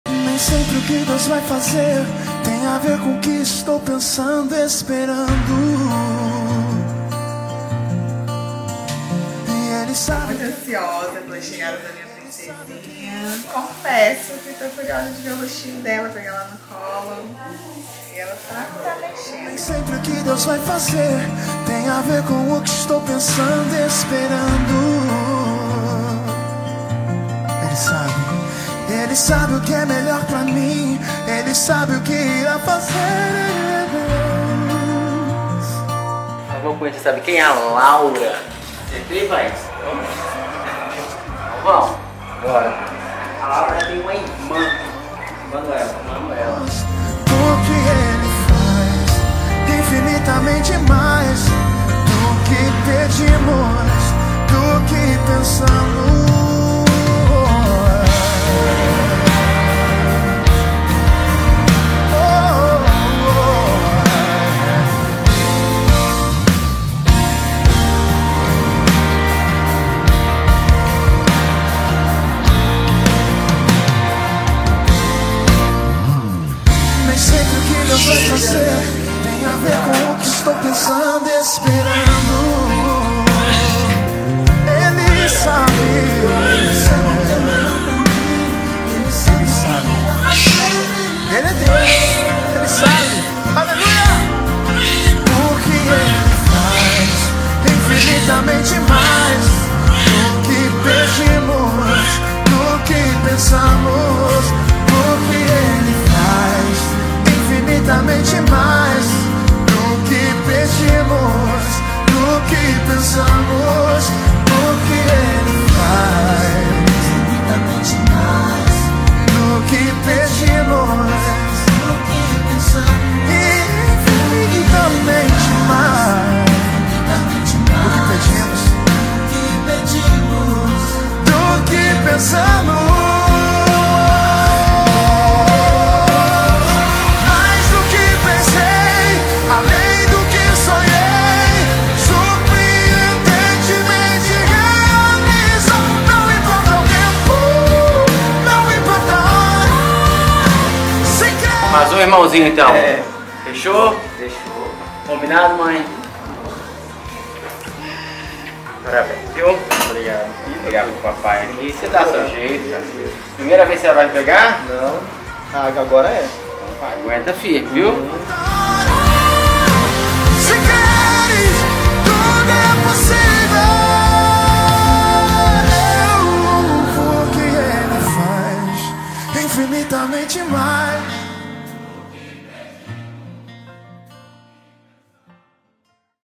NASCIMENTO DA LAURA
Venha se emocionar com o milagre da vida acontecendo.
Orquestrado pelo Obstetra Dr Rafael Angelo e sua equipe, e eternizado pela Paranauê filmes.